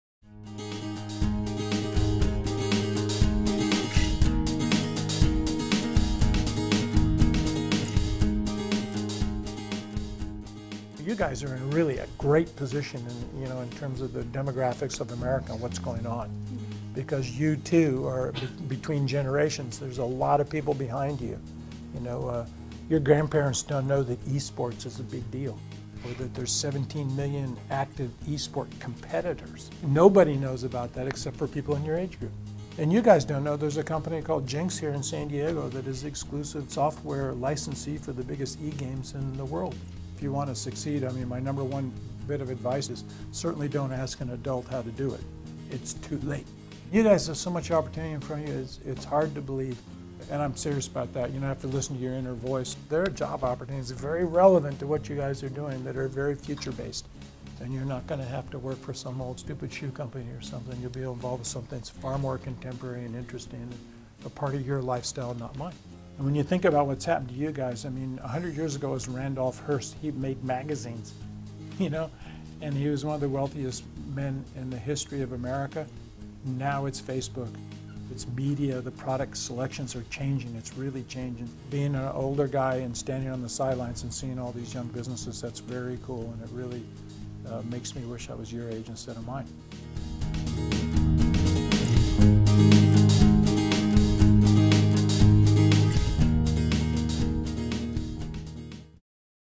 Bob Rief Reminds Millennials of their Value in the Changing Job Market
CEO of San Diego Sport Innovators, Bob Rief highlights the unique position that millennials are in, in regards to the demand shift in the job market at "In the Executive's Chair" -- an open course at CSUSM that invites the community and business students to hear valuable advice from business leaders.

Prior to SDSI, Bob Rief was COO of C4 Waterman and Boardworks Surf, CEO of Sanuk and Reef and GM of Nike Golf and Merrell Hiking Boots.

"In the Executive's Chair" is a series hosted by the College of Business Administration at CSUSM, featuring weekly talks by top executives and CEO's. The series is free and open to the public.